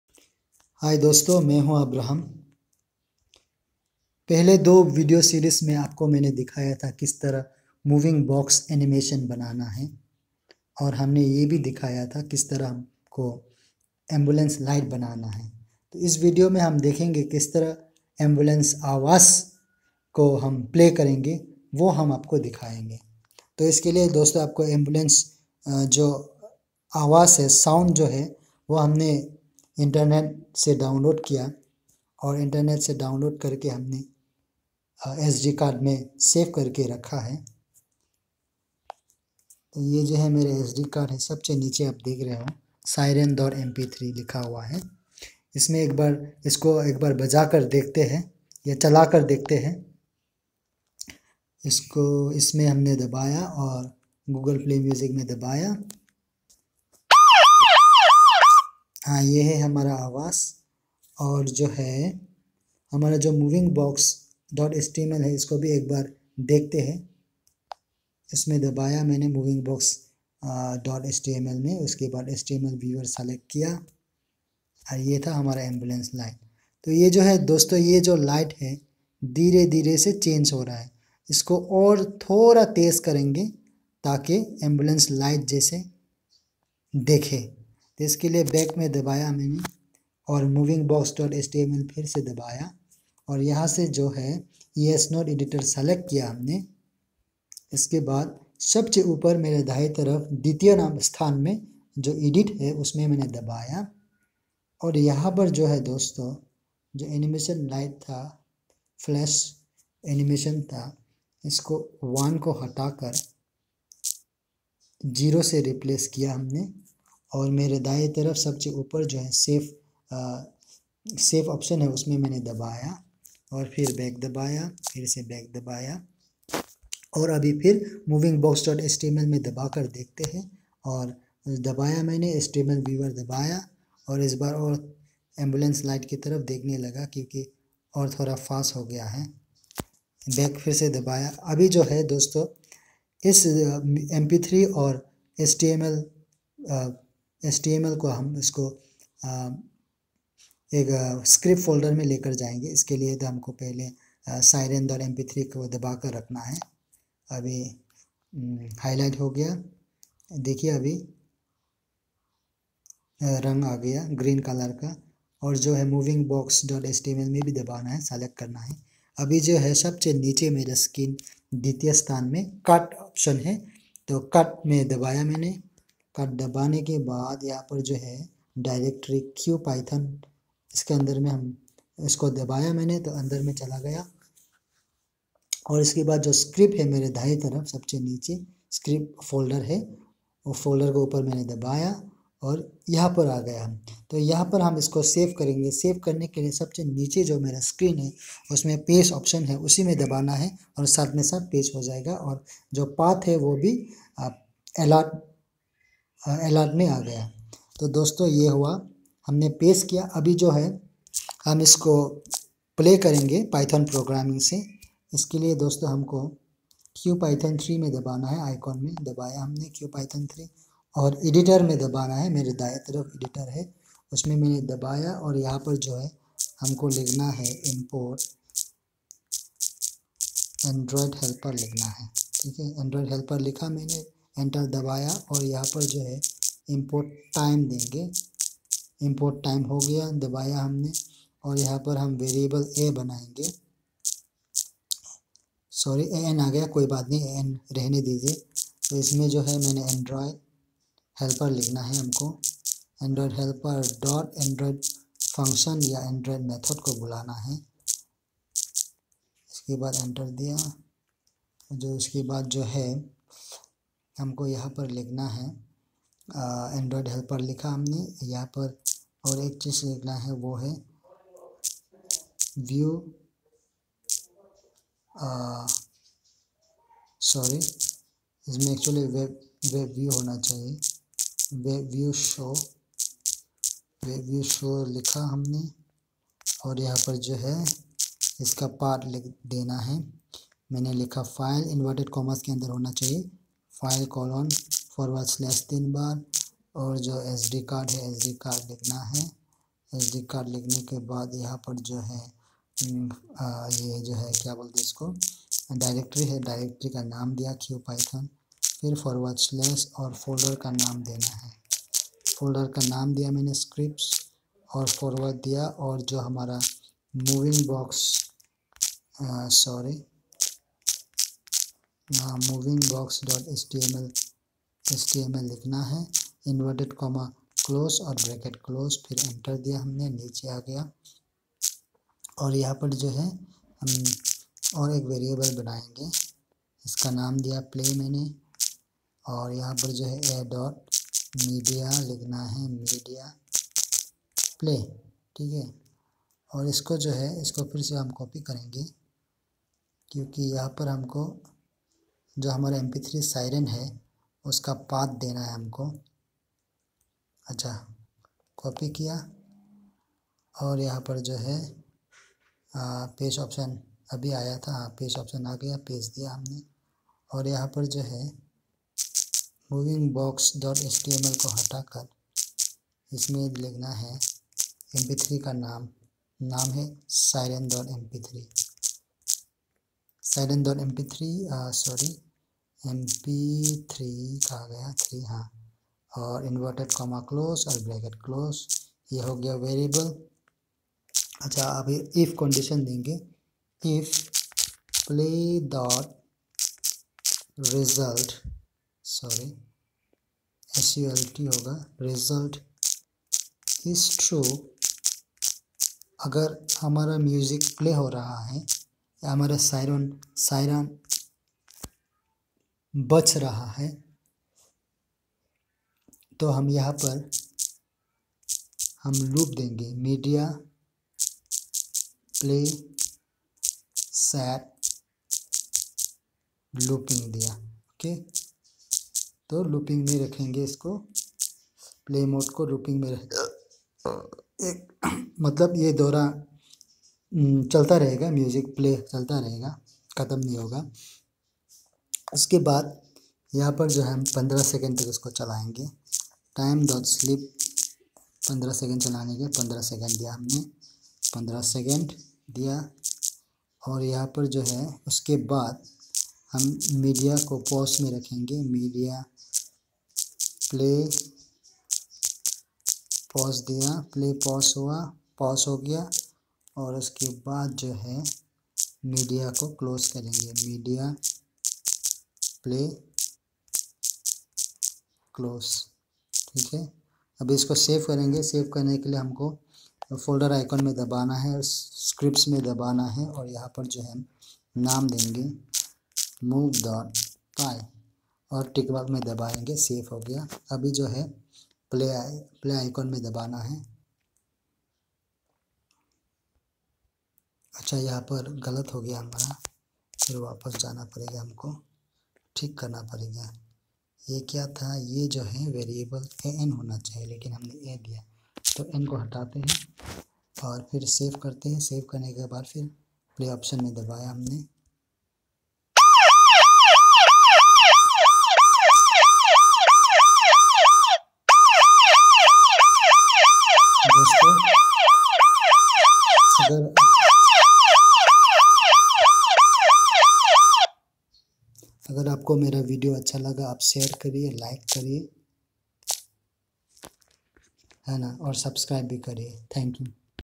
Ambulance Siren
This video tutorial is a continuation of previous video tutorial, "html and Css | keyframes (background-color) | Ambulance Light". In this tutorial I described the technic of using python3 for playing siren.mp3 in the background. Here we are using html and python3 to accomplish our purpose of displaying ambulance light along with background siren.mp3.
I am using webViewShow() function to display html and mediaPlay(src) to play mp3. Both the function are called from main  function android(). Recordedecorder – Screen recorder for Android